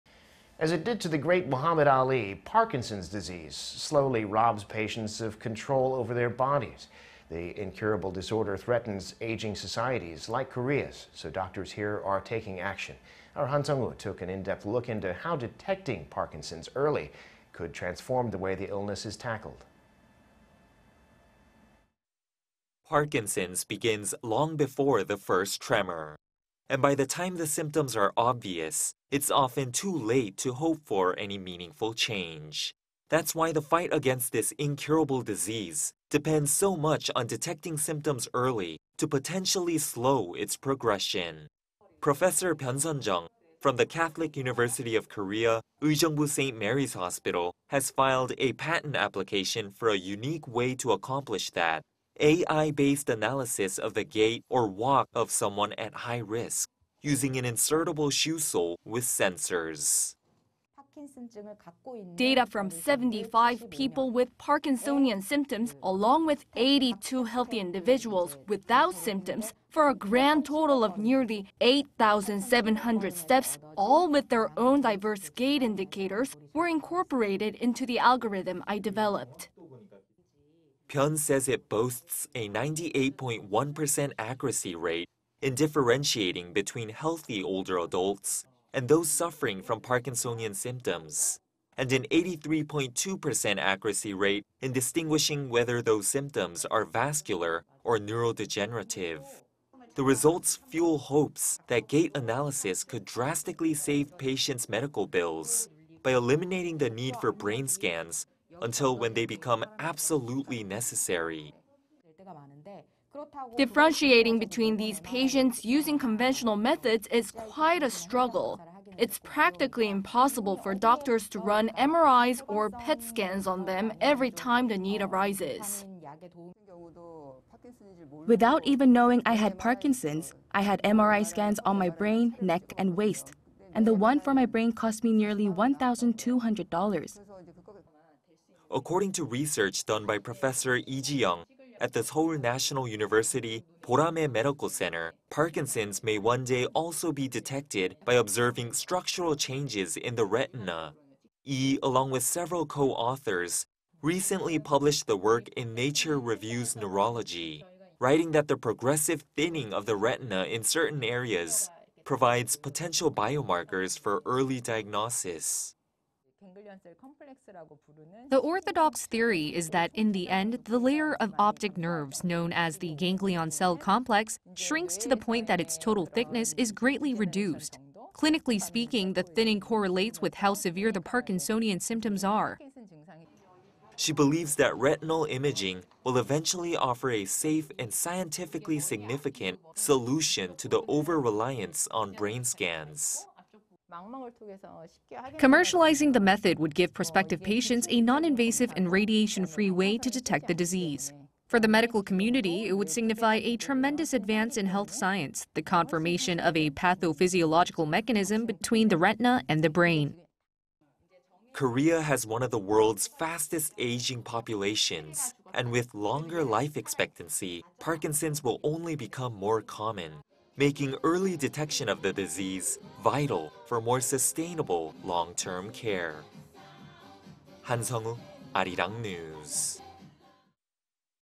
Detecting Parkinson's early: S. Korean doctors analyzing gait and retinal changes as potential...
Detecting Parkinson's early: S. Korean doctors analyzing gait and retinal changes as potential biomarkers for disease
국내 의료진, 걸음걸이와 망막 구조 분석 통해 파킨슨병 조기 판별, 진단 가능성 열어

As it did to the great Muhammad Ali, Parkinson's disease slowly robs patients of control of their body.
The incurable disorder threatens aging societies like Korea, so doctors here are taking action.
Our Han Seong-woo took an in-depth look into how detecting Parkinson's early may transform the way the illness is tackled.
Parkinson's begins long before the first tremor
and by the time the symptoms are obvious, it's often too late to hope for any meaningful change.
That's why the fight against this incurable disease depends so much on detecting symptoms early to potentially slow its progression.
Professor Byun Seon-jeong from the Catholic University of Korea Uijeongbu St. Mary's Hospital, has filed a patent application for a unique way to accomplish that: AI-based analysis of the gait, or walk, of someone at high risk using an insertable shoe sole with sensors.

" data from 75 people with Parkinsonian symptoms along with 82 healthy individuals without symptoms for a grand total of nearly 8,700 steps, all with their own diverse gait indicators were incorporated into the algorithm I developed."

Byun says it boasts a 98.1 percent accuracy rate in differentiating between healthy older adults and those suffering from Parkinsonian symptoms and an 83.2 percent accuracy rate in distinguishing whether those symptoms are vascular or neurodegenerative.
 The results fuel hopes that gait analysis could drastically save patients' medical bills by eliminating the need for brain scans until when they become absolutely necessary.

"Differentiating between these patients using conventional methods is quite a struggle. It's practically impossible for doctors to run MRIs or PET scans on them every time the need arises."

"Without even knowing I had Parkinson's, I had MRI scans on my brain, neck and waist, and the one for my brain cost me nearly 1,200 dollars."

According to research done by Professor Lee Jee-young at the Seoul National University Boramae Medical Center, Parkinson's may one day also be detected by observing structural changes in the retina.
Lee, along with several co-authors, recently published the work in Nature Reviews Neurology writing that the progressive thinning of the retina in certain areas provides potential biomarkers for early diagnosis.

"The orthodox theory is that, in the end, the layer of optic nerves known as the ganglion cell complex shrinks to the point that its total thickness is greatly reduced. Clinically speaking, the thinning correlates with how severe the Parkinsonian symptoms are"

She believes that retinal imaging will eventually offer a safe and scientifically significant solution to the overreliance on brain scans.

"Commercializing the method would give prospective patients a non-invasive and radiation-free way to detect the disease. For the medical community, it would signify a tremendous advance in health science: the confirmation of a pathophysiological mechanism between the retina and the brain."

Korea has one of the world's fastest aging populations, and with longer life expectancy, Parkinson's will only become more common making early detection of the disease vital for more sustainable, long-term care.
Han Seong-woo, Arirang News."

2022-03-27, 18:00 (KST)